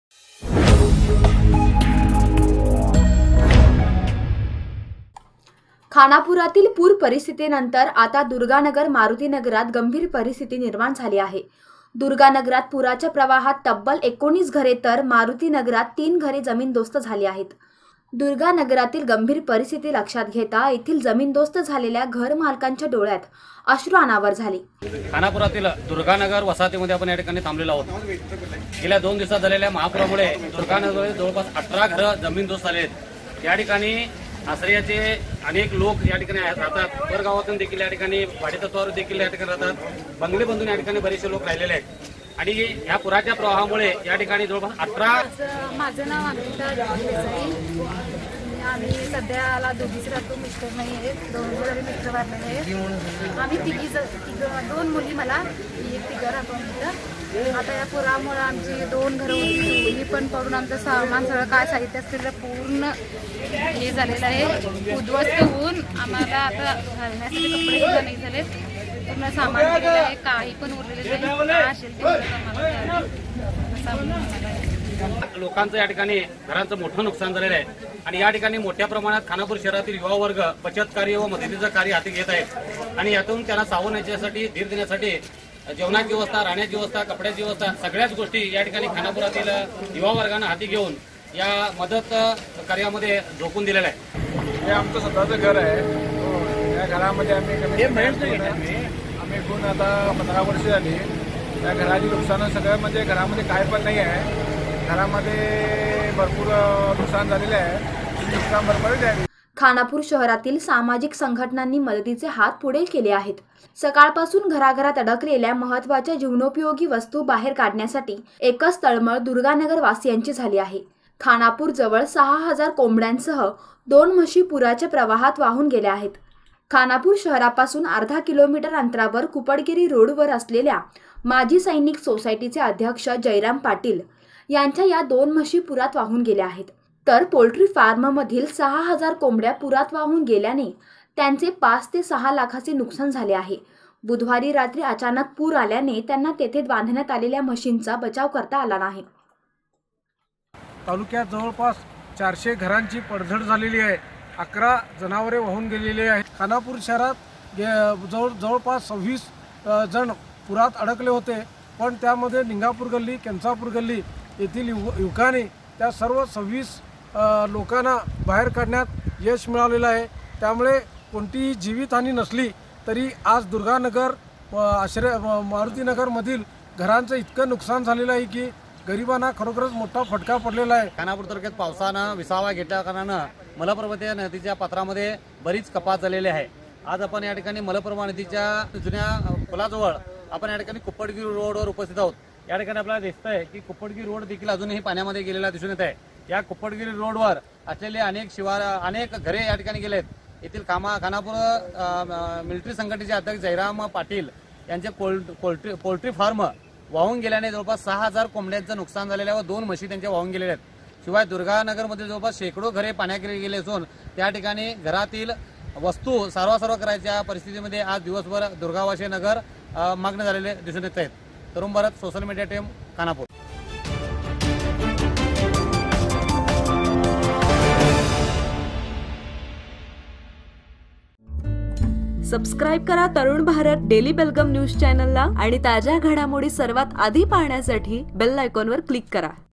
पोल्ट्री फार्म मधील 6000 कोंबडय़ा पुरात वाहून गेल्या| Khanapur News | 10-08-2019
खानापुरातील पूरपाfरस्थिती नंतर दुर्गा नगर मारुती नगरात गंभीर पाfरस्थिती निर्माण झाली आहे. दुर्गा नगरात पुराच्या प्रवाहात तब्बल 19 घरे तर मारुती नगरात तीन घरे जमीनदोस्त झाली आहेत.